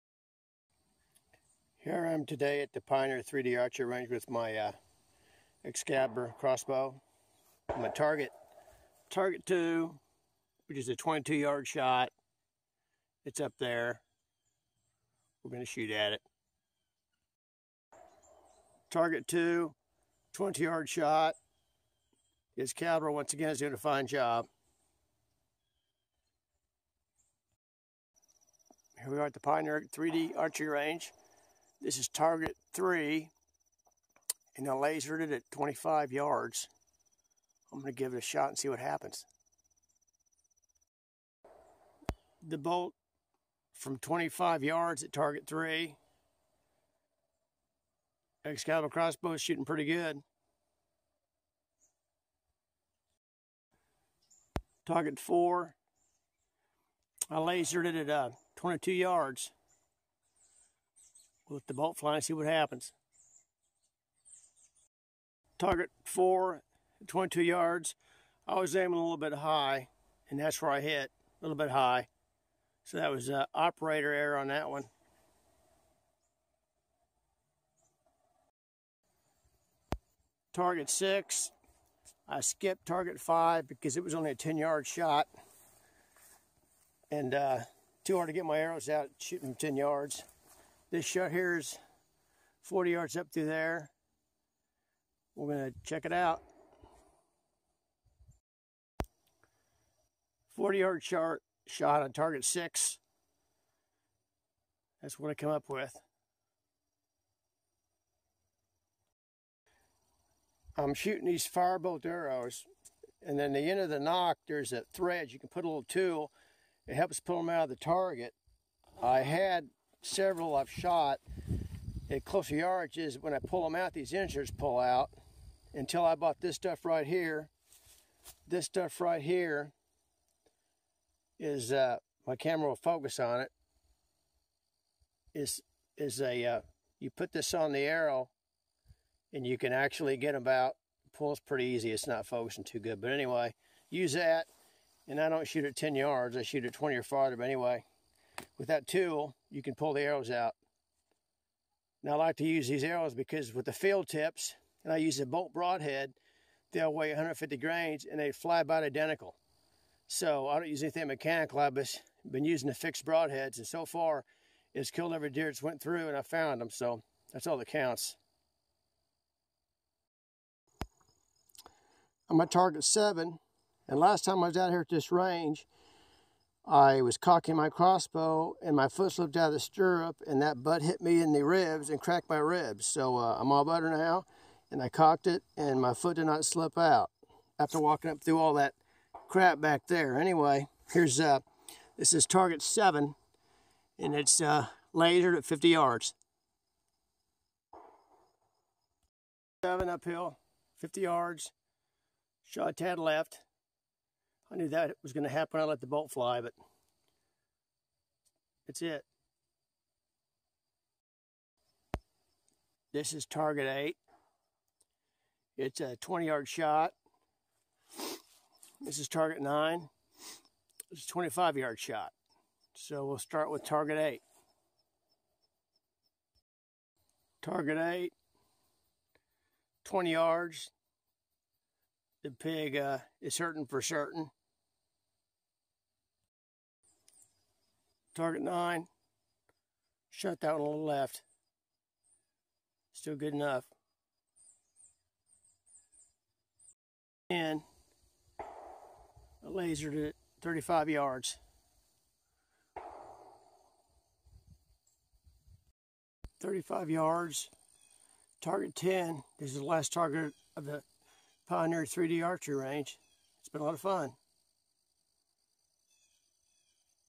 pioneer 3d archeryFull HD 10/9/2015
Practicing at the Pioneer 3D Archery Range with my Excalibur Axiom crossbow, it has a Excalibur Tach Zone Scope, I am shooting Excalibur Firebolt Bolts with 150 gr. field tips, they fly about identical to the Excalibur Bolt 150 gr. fixed broadheads I use.